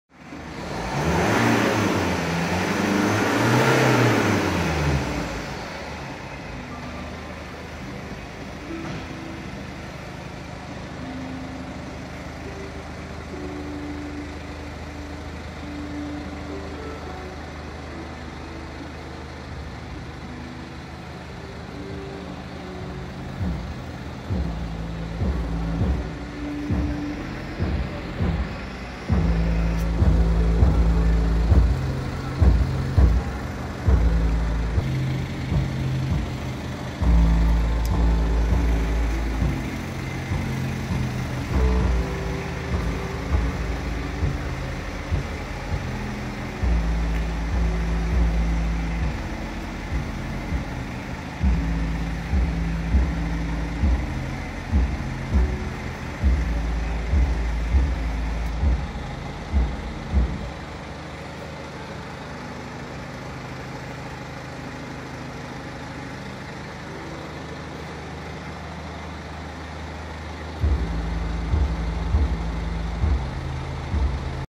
BMW 320
Продається машина 2014 року. Ціна
19000 євро.